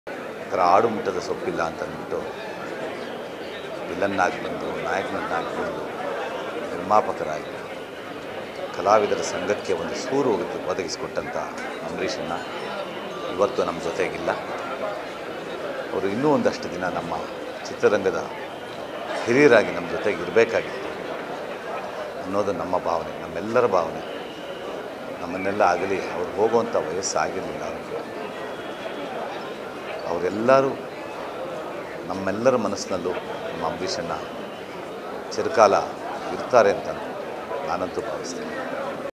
‘ನಮ್ಮೆಲ್ಲರ ಮನಸ್ಸಲ್ಲಿ ಅಂಬರೀಶಣ್ಣ ಚಿರಕಾಲ ಇರ್ತಾರೆ’ | Kari Subbu | FIRSTNEWS
‘ನಮ್ಮೆಲ್ಲರ ಮನಸ್ಸಲ್ಲಿ ಅಂಬರೀಶಣ್ಣ ಚಿರಕಾಲ ಇರ್ತಾರೆ’ | Kari Subbu| Rebel Star Ambareesh no more

FirstNews Kannada Karnataka's youngest and liveliest news channel